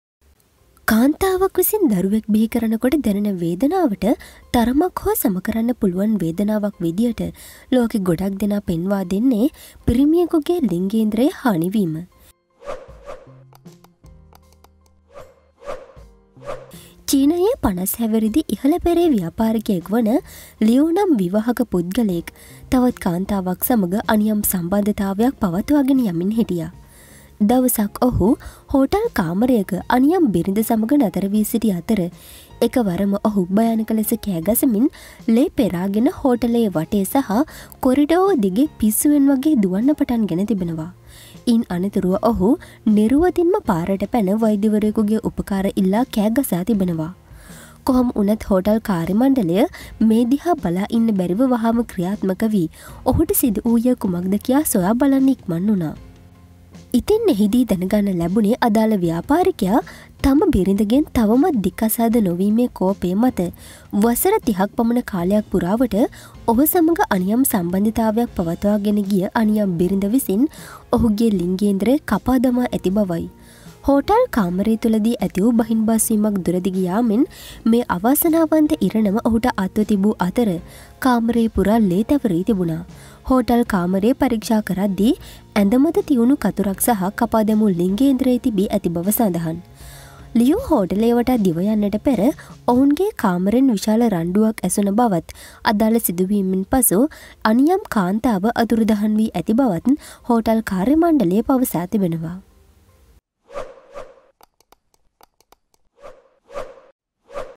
ලිංගේන්ද්‍රය කපා පලිගත්පෙම්වතිය
කාන්තාවක් විසින් දරුවෙක් බිහිකරනකොට දැනෙන වේදනාවට තරමක් හෝ සමකරන්න පුලුවන් වේදනාවක් විදියට ලෝකෙ බොහෝ දෙනා පෙන්වා දෙන්නේ, පිරිමියෙකුගේ ලිංගේන්ද්‍රය හානි වීමයි

චීනයේ 50 හැවිරිදි ඉහල පෙලේ ව්‍යාපාරිකයෙක් වන, ලියූ නම් විවාහක  පුද්ගලයෙක්, තවත් කාන්තාවක් සමග අනියම් සම්බන්ධයක් පවත්වාගෙන යමින් හිටියා.